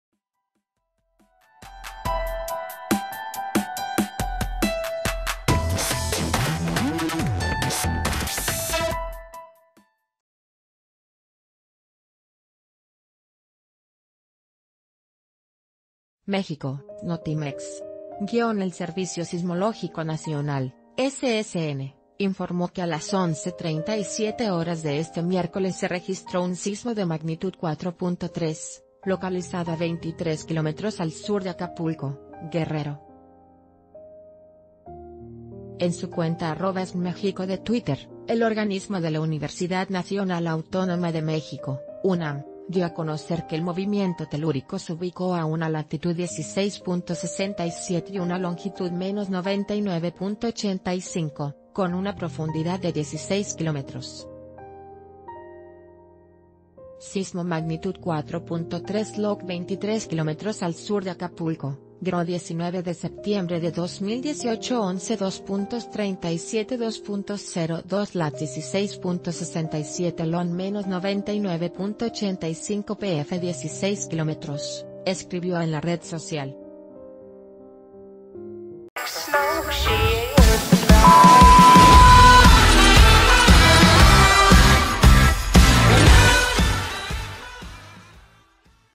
Sismo de magnitud 4.3 sacude Acapulco - El Diario de Yucatán
MÉXICO  (Notimex).- El Servicio Sismológico Nacional (SSN) informó que a las 11:37 horas de este miércoles se registró un sismo de magnitud 4.3, localizado a 23 kilómetros al sur de Acapulco, Guerrero. En su cuenta @SSNMexico de Twitter, el organismo de la Universidad Nacional Autónoma de México (UN...